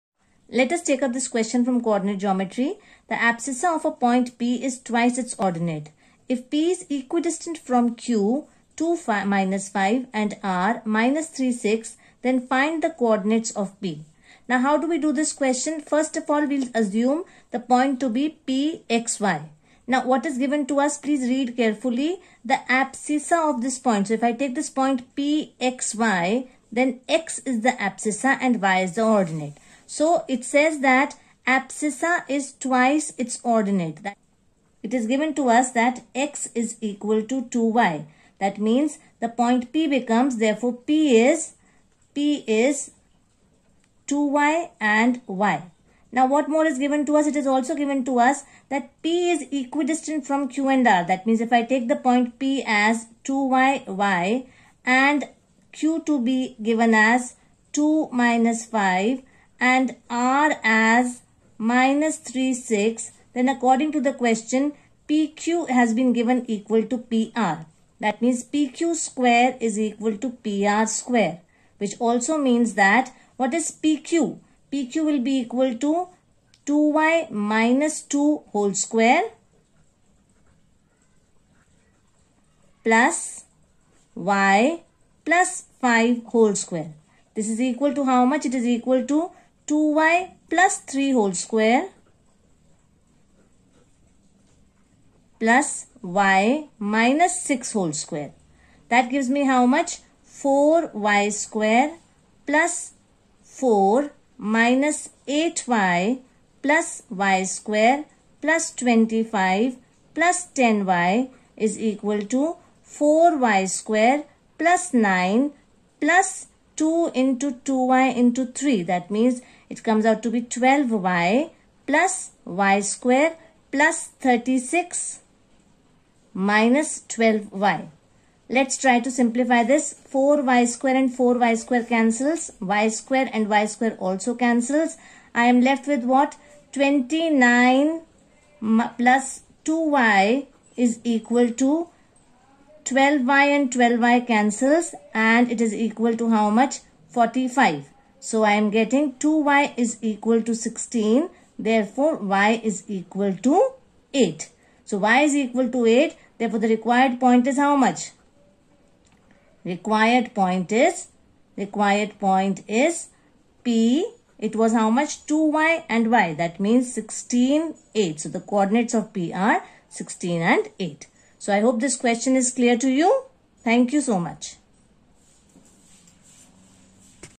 COORDINATE GEOMETRY : The abscissa of a point P is twice its ordinate. If P is equidistant........
The abscissa of a point P is twice its ordinate. If P is equidistant from Q (2,- 5) and R(-3, 6),
then find the coordinates of P